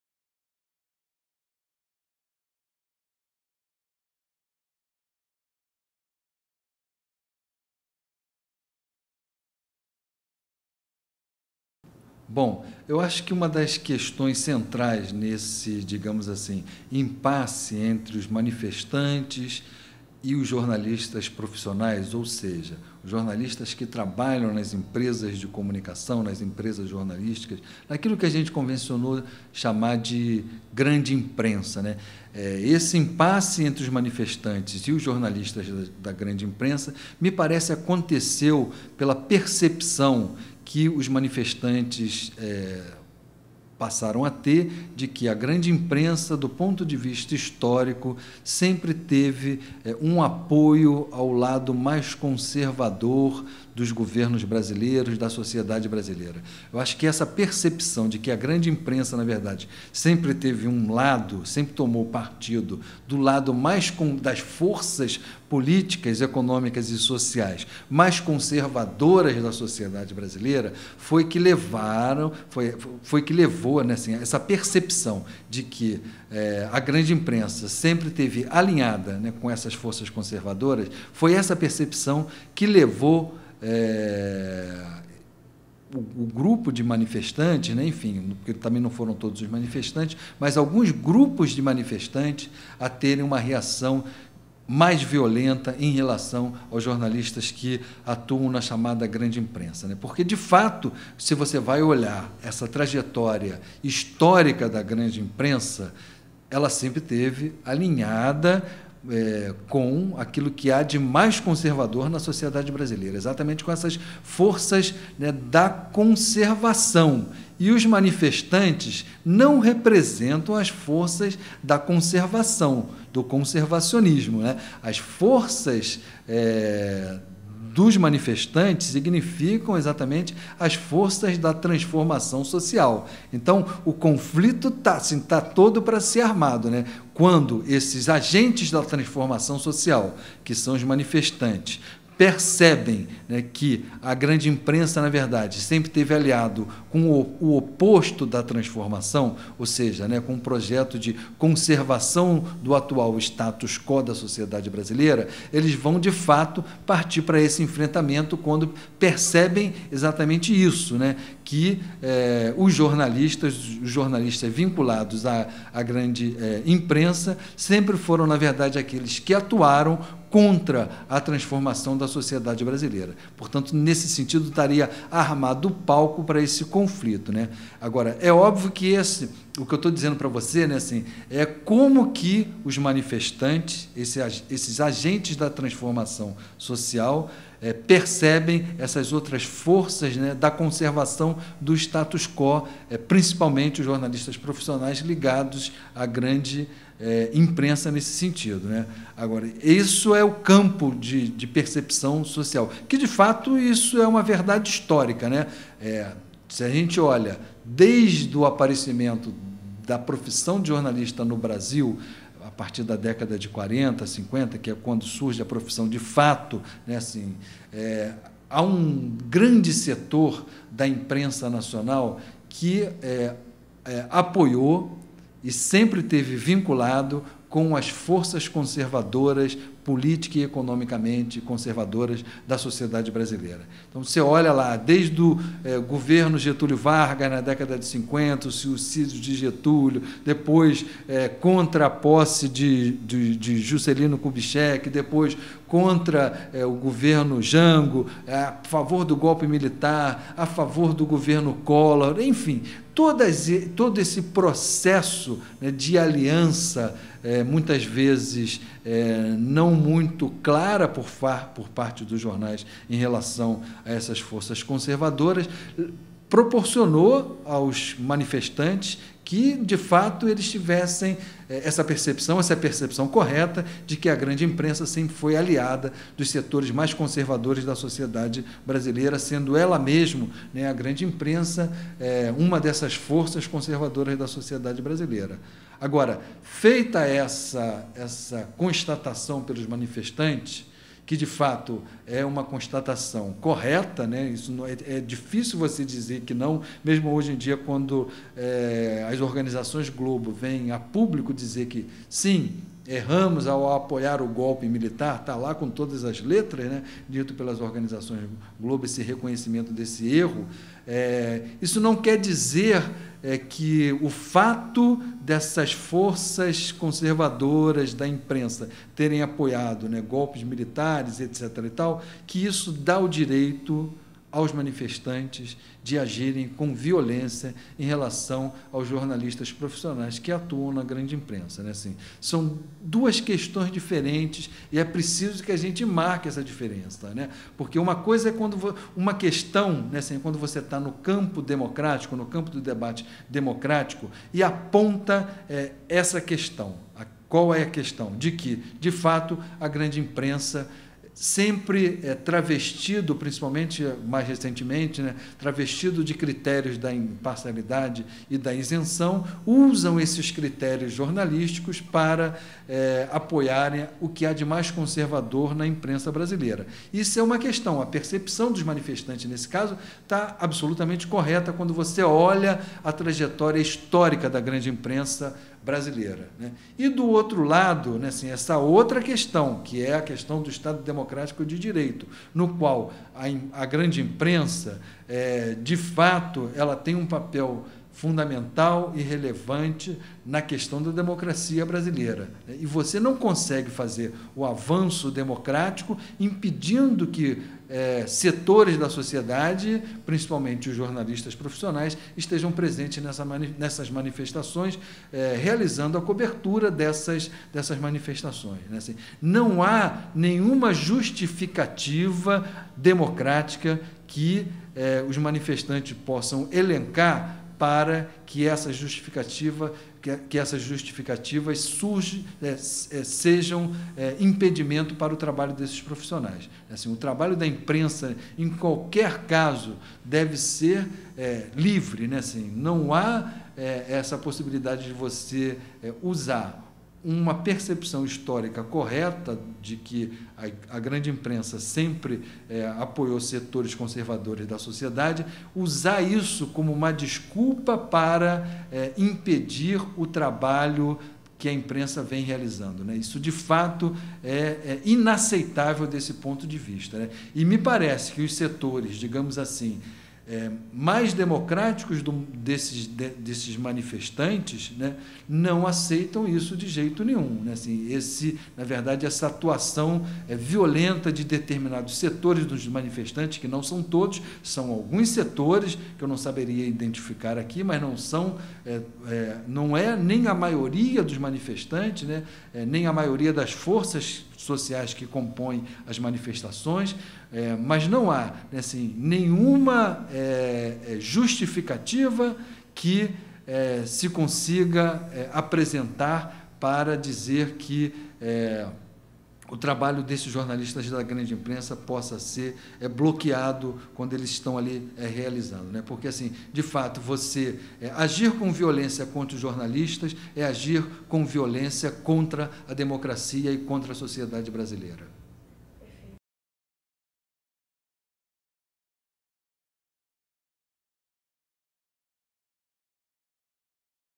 TV PUC Rio - A Imprensa nas Manifestações II
O professor Leonel Aguiar dá a sua opinião sobre o processo de isolamento da mídia pela sociedade brasileira nos últimos meses.